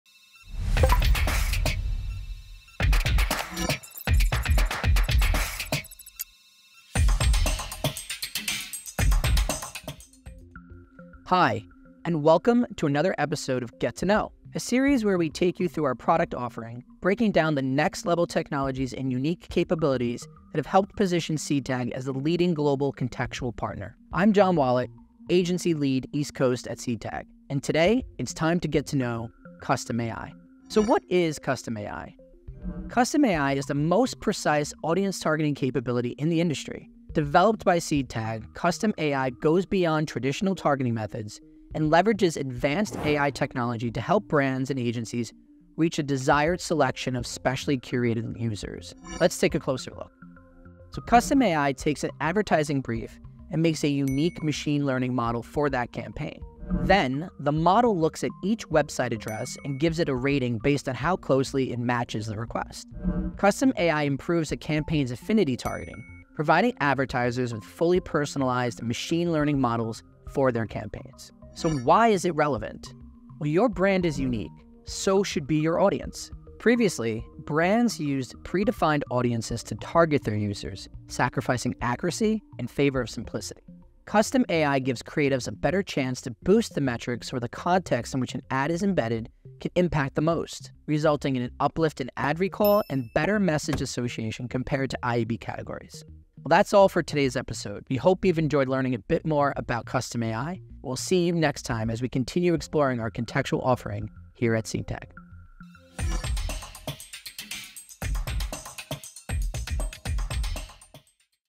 Get to Know: Custom AI - What is Custom AI?
How can brands & agencies masterfully engage a precisely curated consumer base? 🎯

It’s time to ‘Get to Know’ Custom AI.

In this series, we break down the next-level technologies & unique capabilities that have helped position Seedtag as the leading global contextual partner. 🚀

First up, in part one of a two-part episode, Jon Wallet, Vice President of East Coast Sales at Seedtag, will take us through the basics of Custom AI, the most precise audience targeting capability that goes beyond traditional targeting methods.